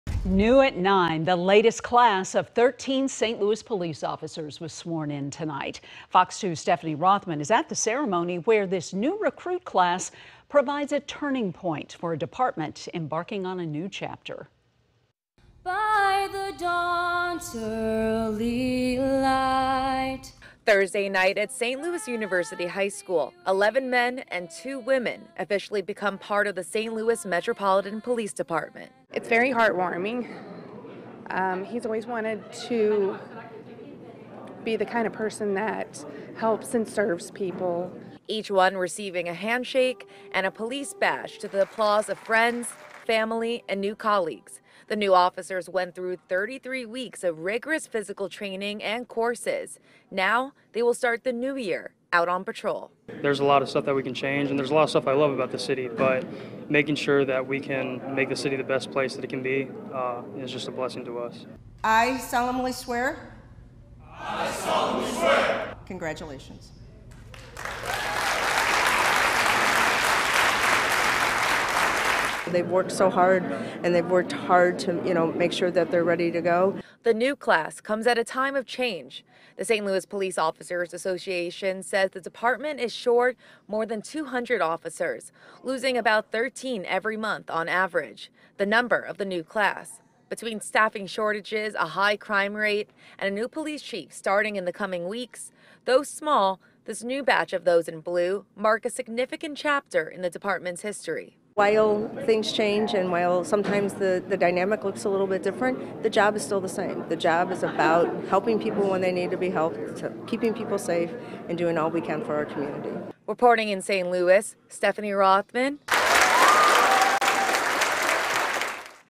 Thirteen recruits sworn in as latest class of St. Louis police officers
Thirteen new recruits were sworn in as the 2022-02 class of police officers for the St. Louis Metropolitan Police Department.